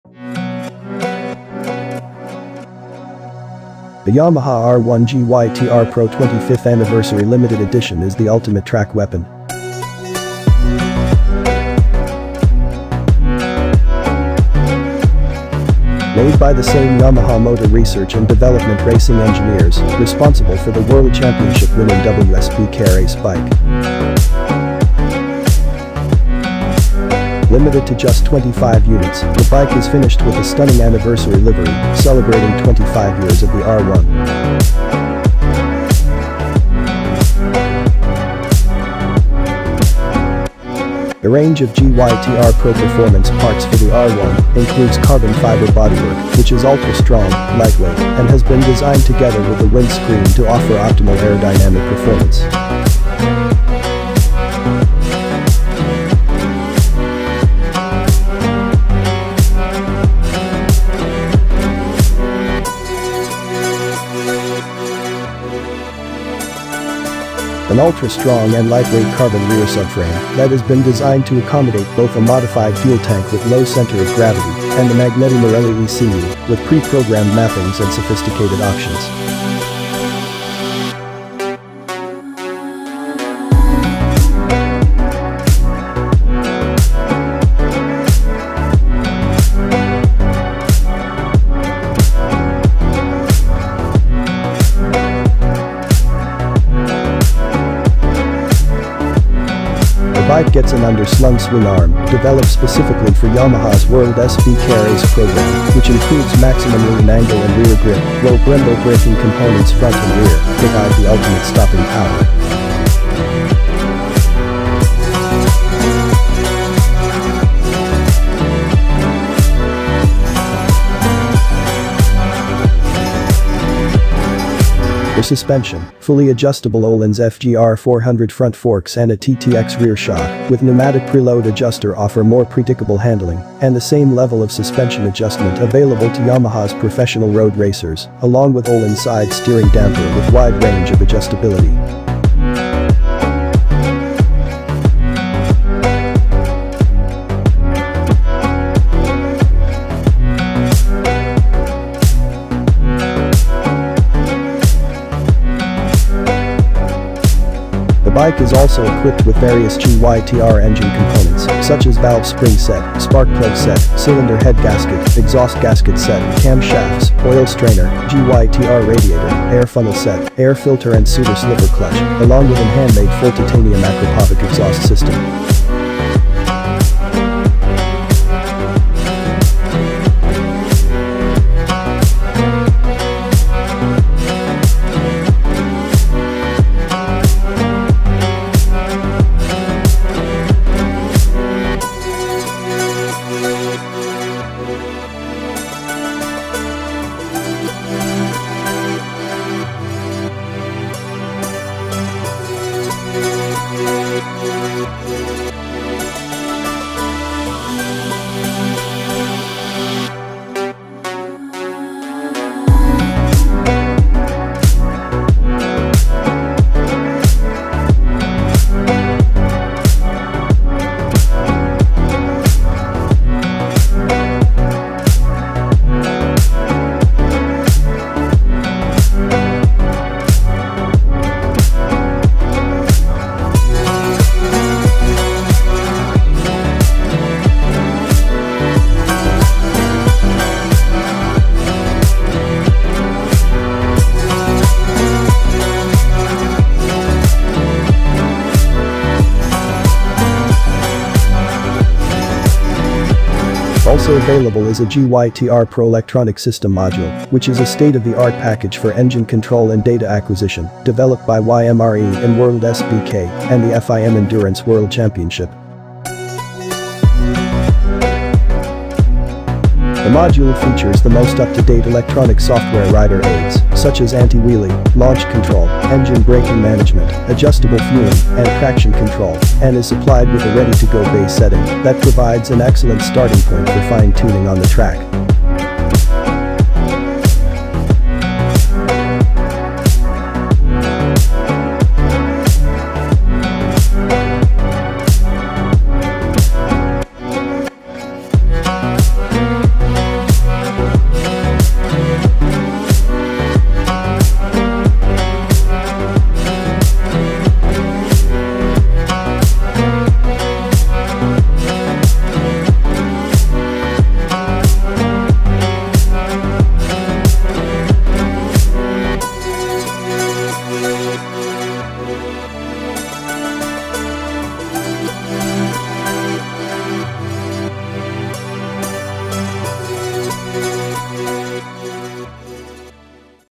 Yamaha R1 GYTR PRO 25th Anniversary Limited Edition 🔥 Ultimate Track Bike
YAMAHA YZF-R1 GYTR TRACKDAY KIT

- GYTR Akrapovic trackday muffler + link pipe;
- GYTR adjustable racing rearset;
- GYTR steering stopper;
- GYTR keyless fuel cap;
- GYTR stainless steel brake lines;
- GYTR ABS emulator;
- GYTR brake reservoir protection;
- GYTR brake lever guard;
- GYTR rear sprocket guard (shark fin);
- GYTR 520 chain/sprocket conversion;
- Motocarbons complete fairing set;
- GYTR race seats seat foam;
- GYTR R1 tank grips;
- Pirelli Superbike SC1 slicks;
- TKRP 90 degree tyre valve set;
- GYTR paddock stand hooks;
- GYTR rear paddock stand.